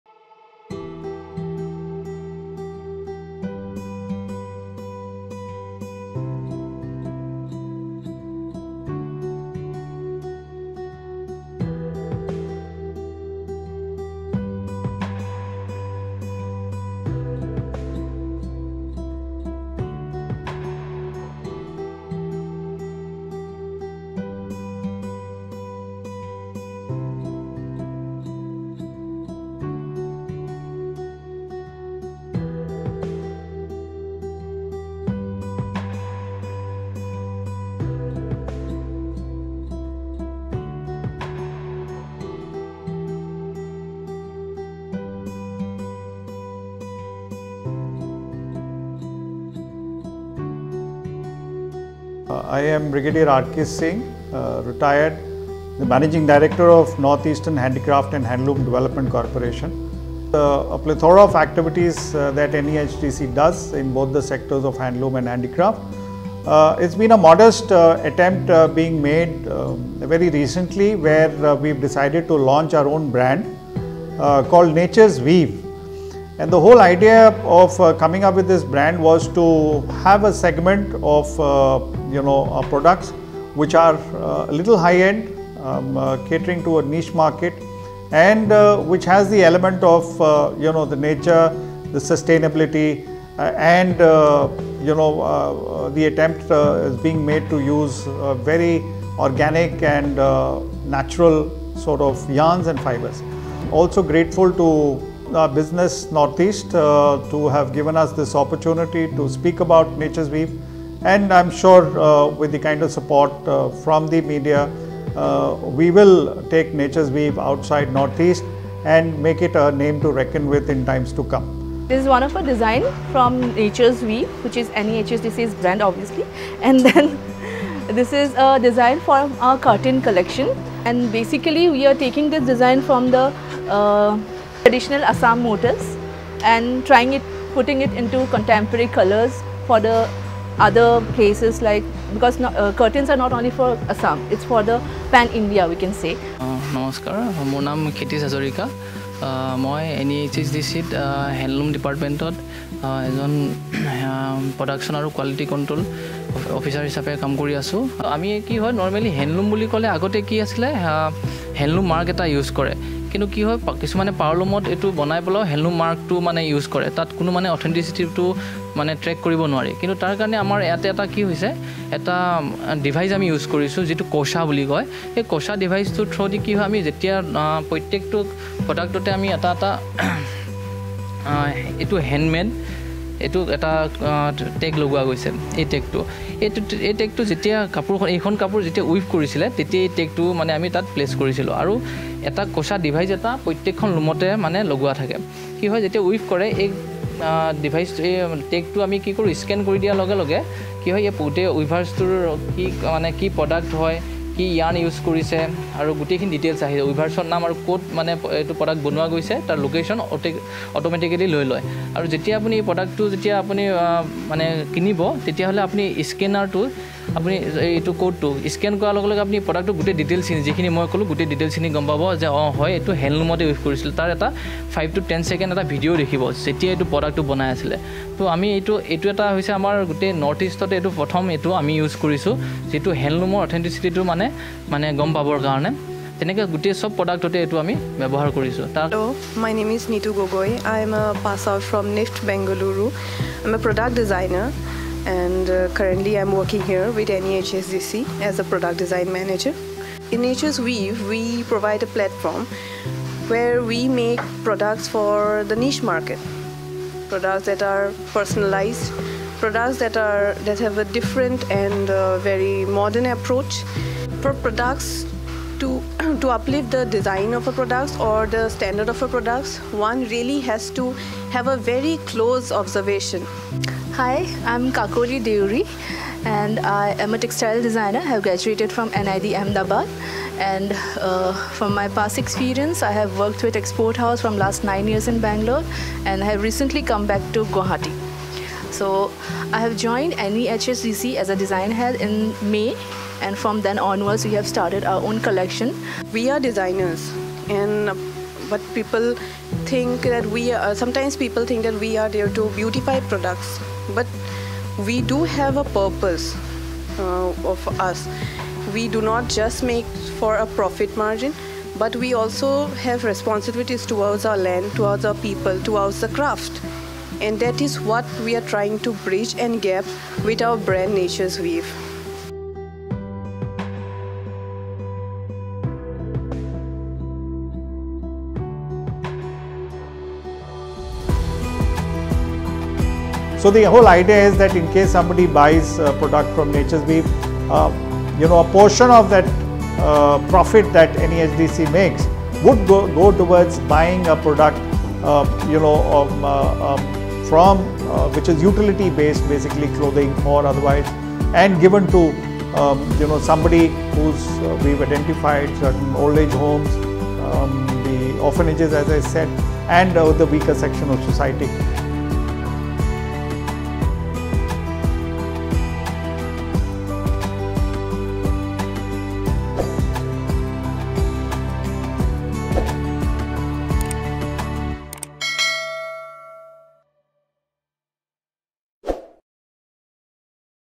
Nature's Weave- NEHHDC's in house brand
Nature's Weave- NEHHDC's in-house is finally launching. The products are sarees and home utility products like lamp shades, table runners and others. All the products are handloom and handicraft which will cater to pan-Indian audiences.
To Read further, Check out
Follow our Facebook Page for more updates ~
Follow us for more updates @_businessnortheast_